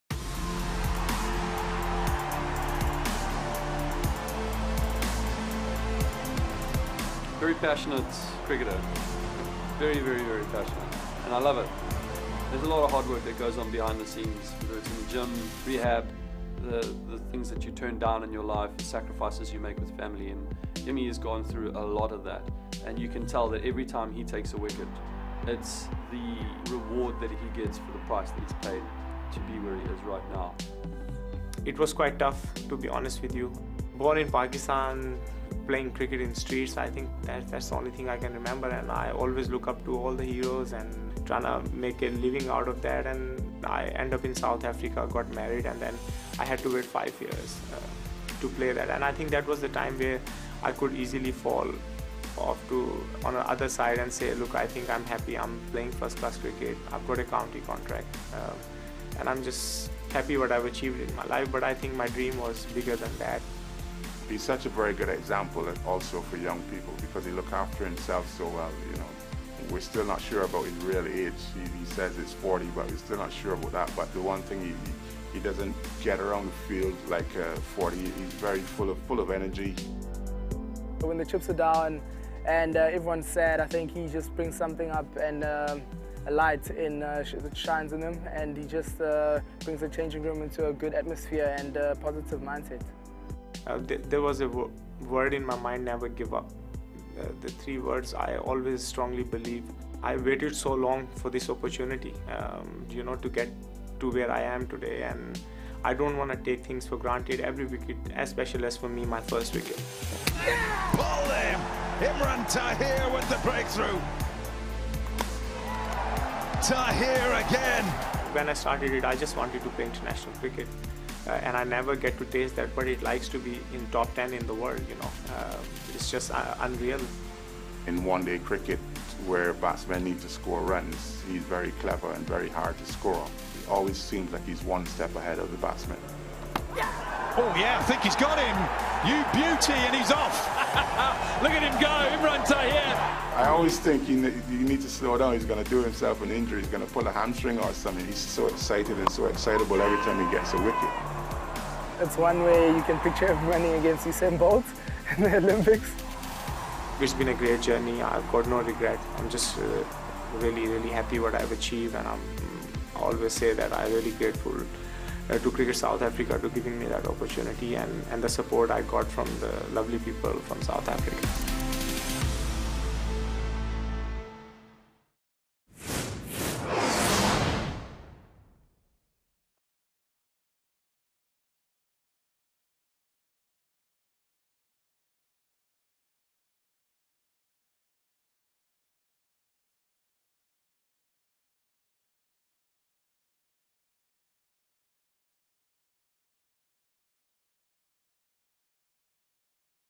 "Never Give Up" - Imran Tahir's Journey To The Top | SA Player Feature | ICC Cricket World Cup 2019
Imran Tahir's rise to the top of the game has not always been easy, and his teammates reveal how his attitude always drove him forward.